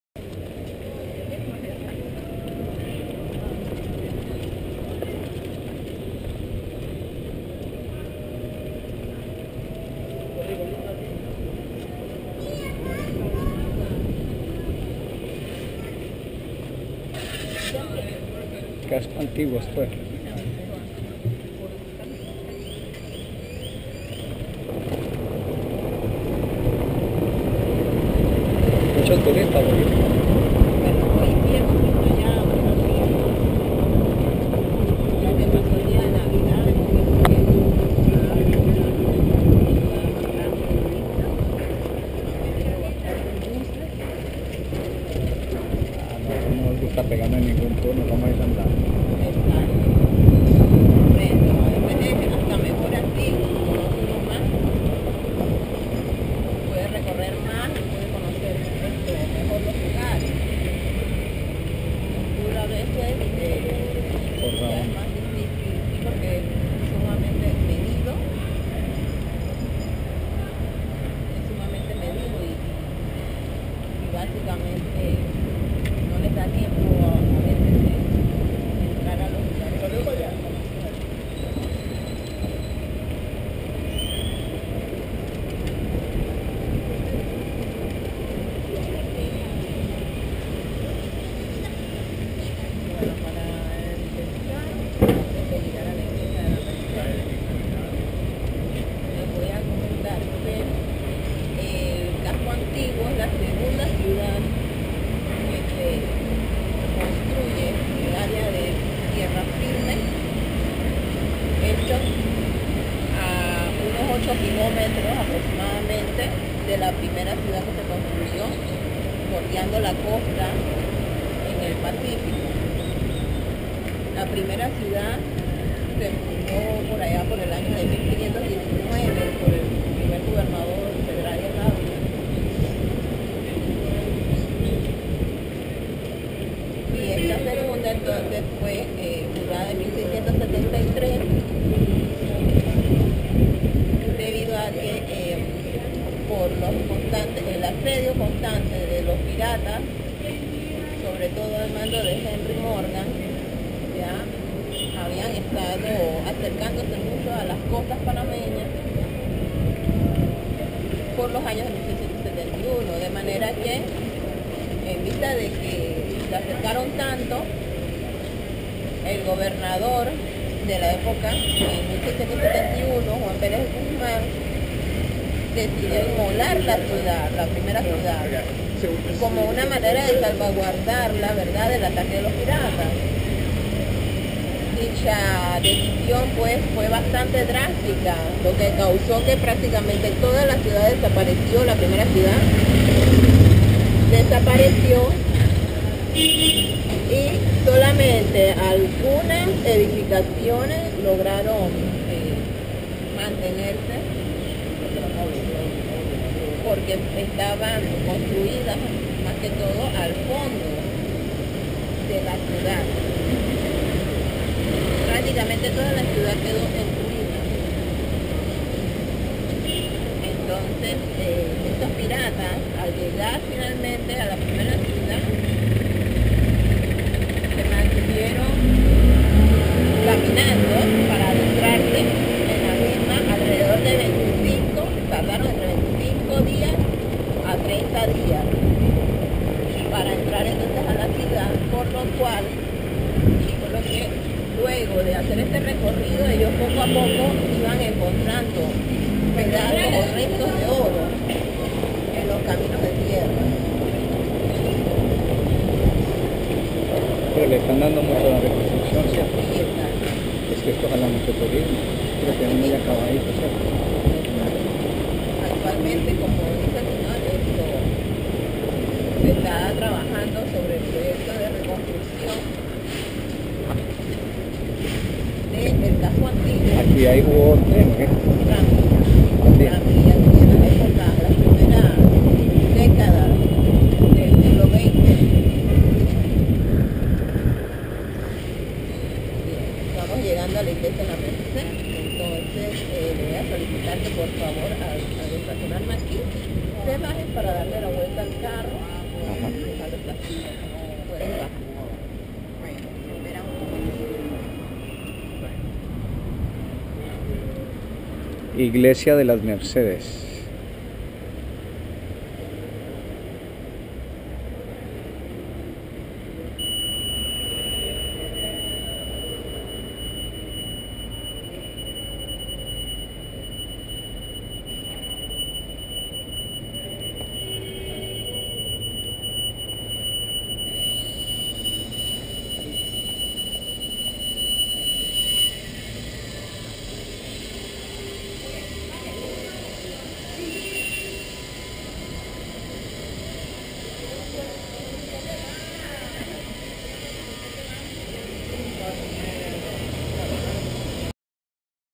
Palacio Bolivar Casco Antiguo Panama
Casco antiguo es la segunda ciudad que se construye en tierra firme a unos 8 Kilometros de la primera ciudad que se construyo en Panamá Antigua, paso por el Ministerio de Relaciones Exteriores Palacio Bolivar, Escuela Simón Bolivar, Escuela República de México, iglesia de las Mercedes.